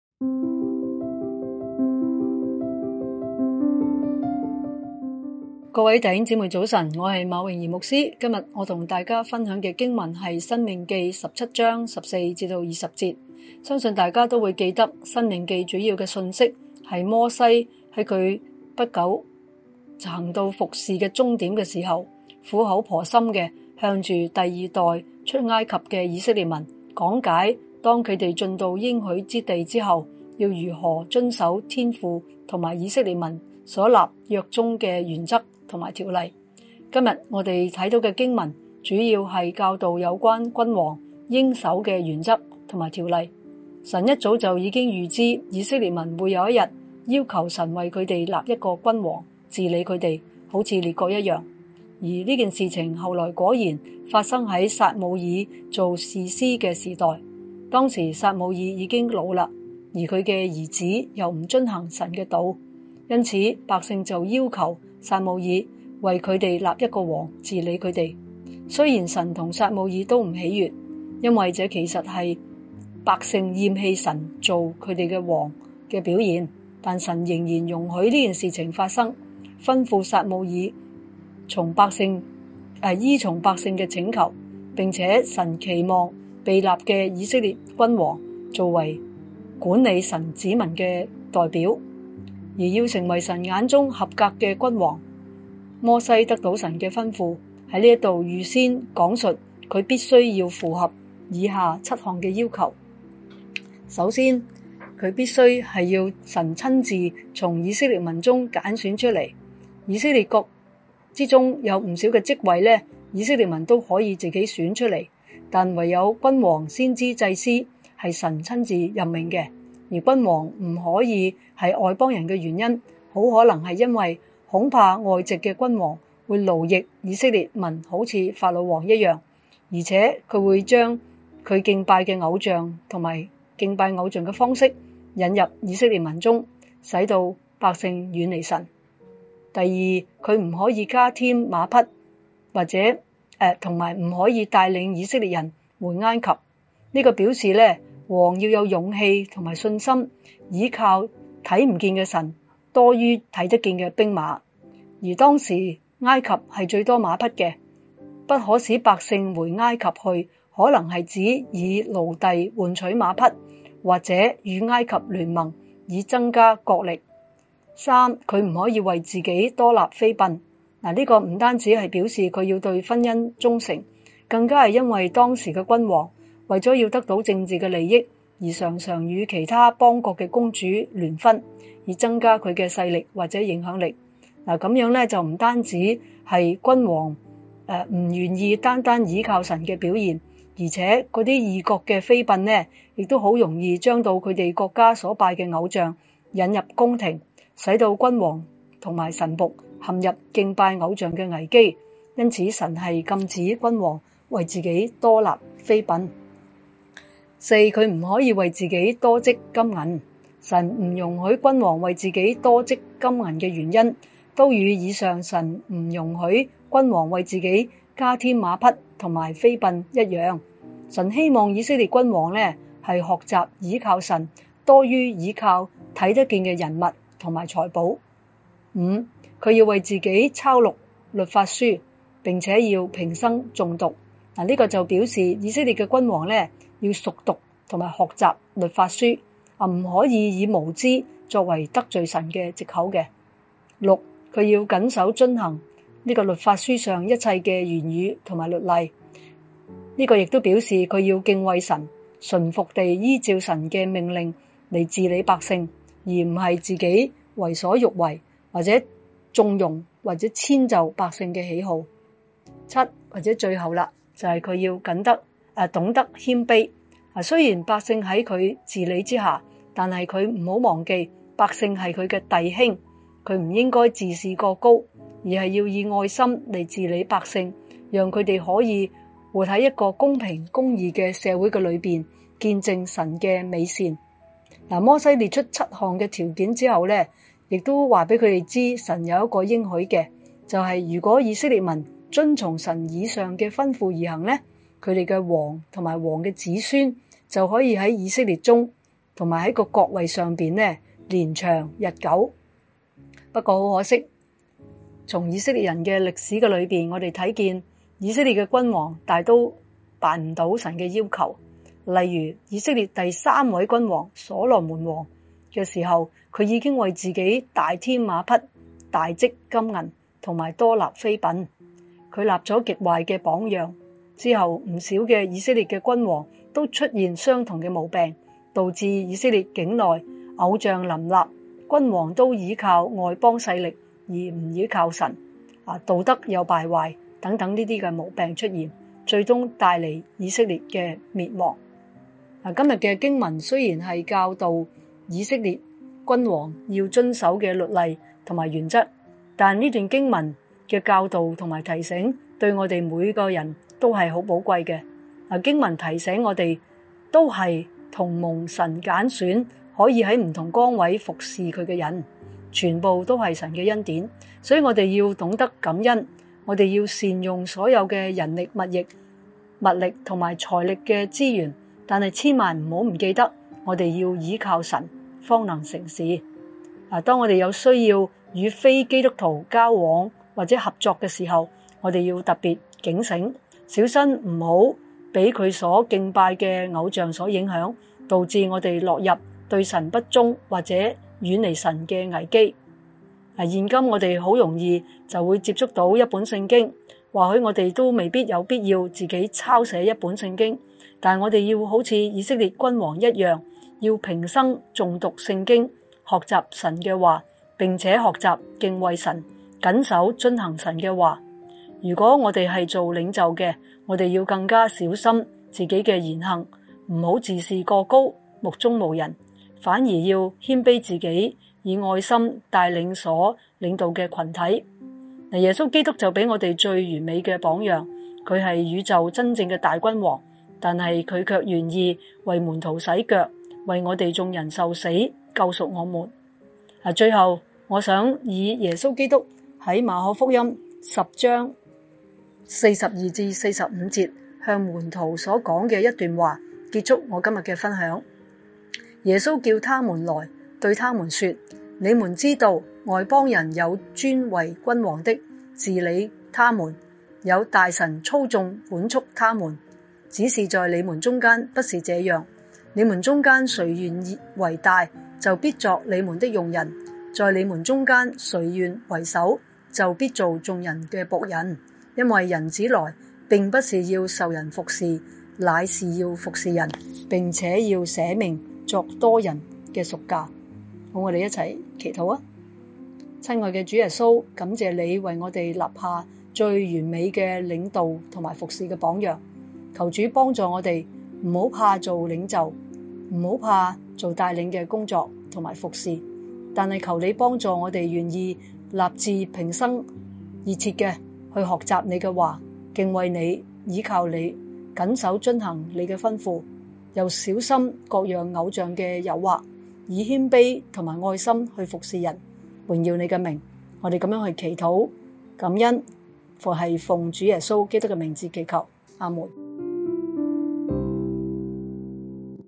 20240401城浸早晨，樂享聖言
分享：馬穎儀牧師
經文：申命記17章14-20節
信息文字版及經文：